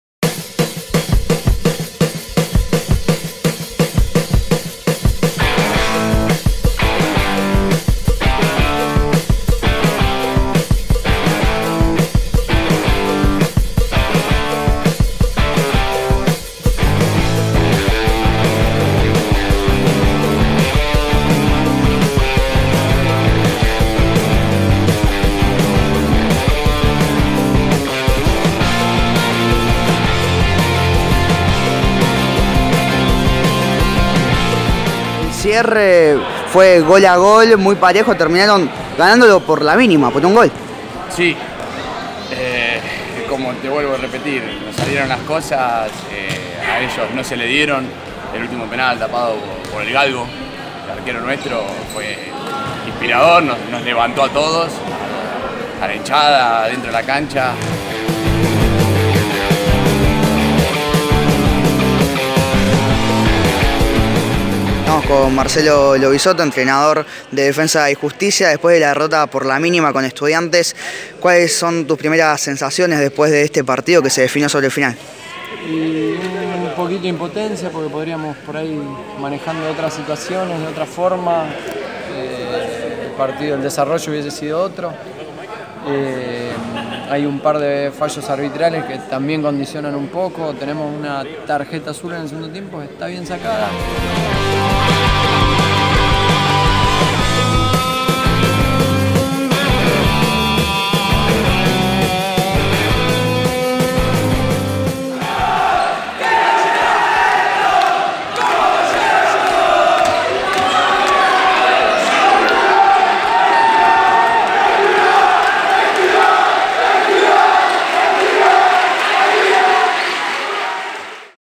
PrimeraCaballeros Fecha 11 | Defensa y Justicia vs Estudiantes de La Plata
En una nueva edición del clásico entre Halcones y Pinchas en La Caldera, el visitante se llevó los tres puntos en los últimos minutos. En el resumen podrás ver las mejores acciones del partido y las declaraciones de los protagonistas.